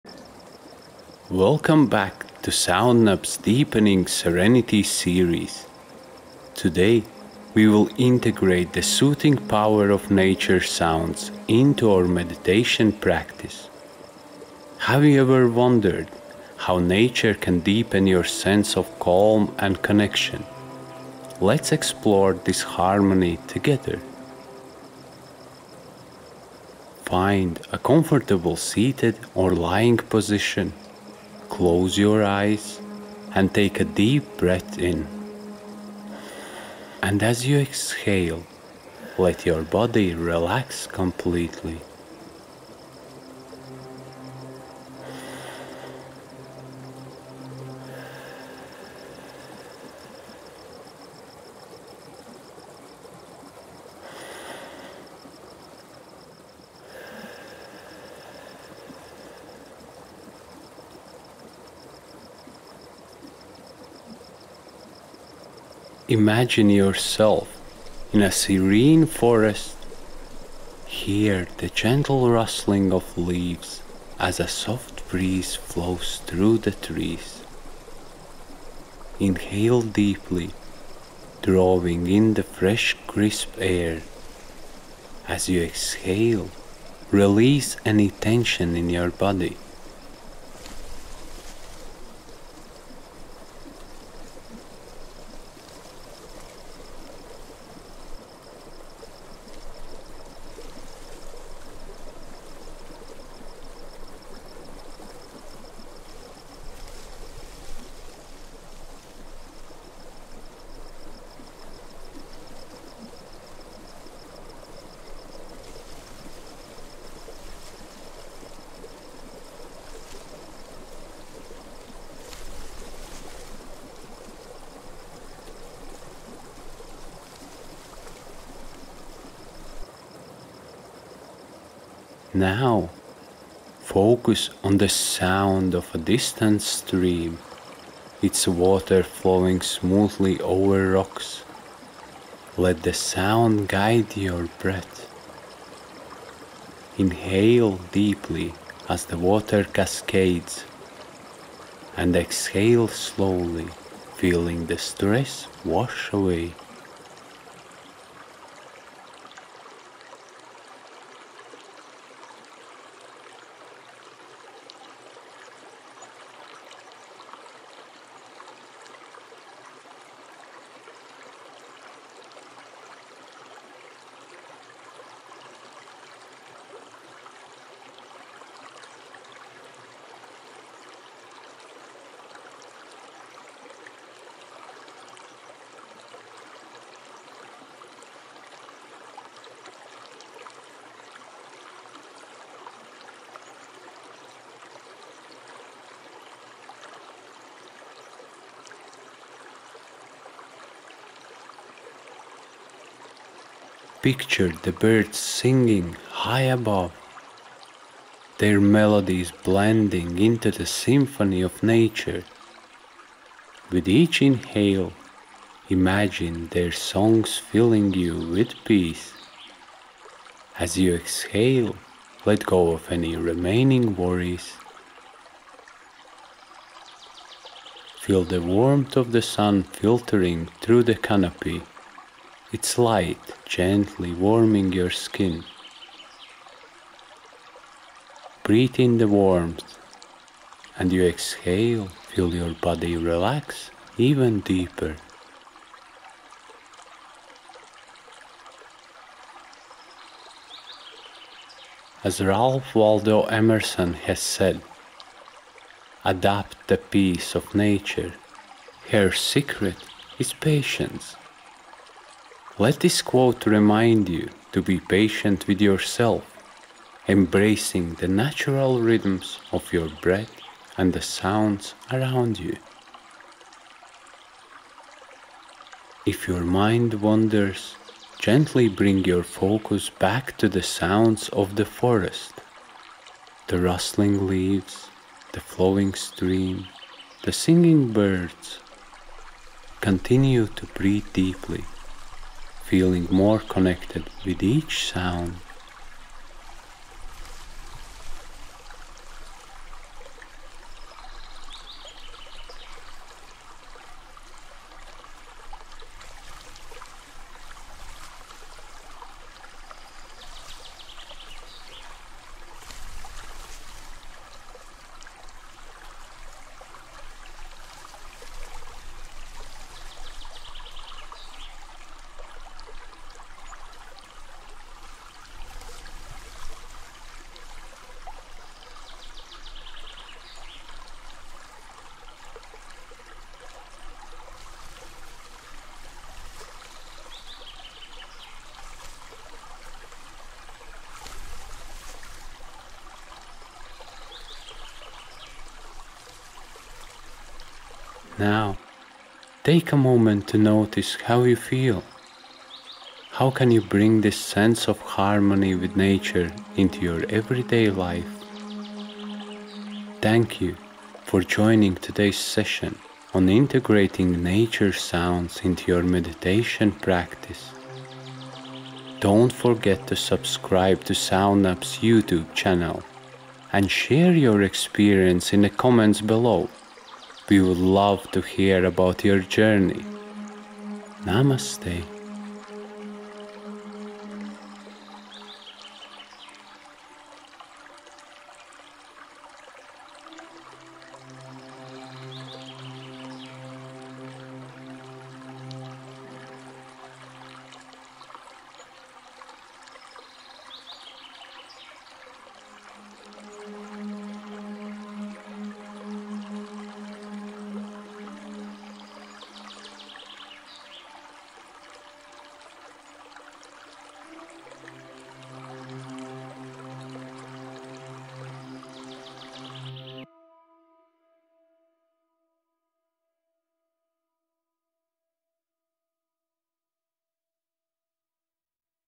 Guided Meditation: Nature Sounds Integration "Harmony with Nature" 🌳🎶🌿🌺
🌟 Welcome back to Week 3 of our "Deepening Serenity: 12 Weeks of Transformative Meditation" series! 🌟

In this session, titled "Harmony with Nature," we integrate the soothing sounds of nature into our meditation practice. 🌲✨ Immerse yourself in the calming symphony of rustling leaves, flowing streams, and birdsong, guiding you to a deeper sense of peace and connection. 🌿🌞

Reflect on how you can bring this harmony into your everyday life. 🧘‍♂️🧘‍♀️

Video made with FL Studio ZGameEditor Visualizer plugin  Effect author credits:  Dubswitcher Soft Glitch, Dubswitcher Whitehole, Dubswitcher Linear Blur – Dubswitcher

Have you already heard our relaxation music?